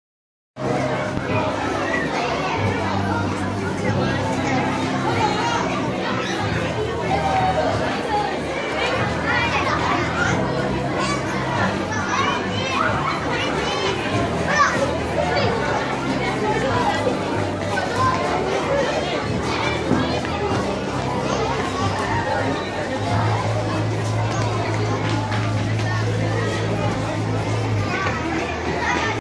Joel, Jason, Joseph, Nunui te school
Jason, Joseph te school banah an inkawm lai. Neuhof School an kallai.
22nd November 2016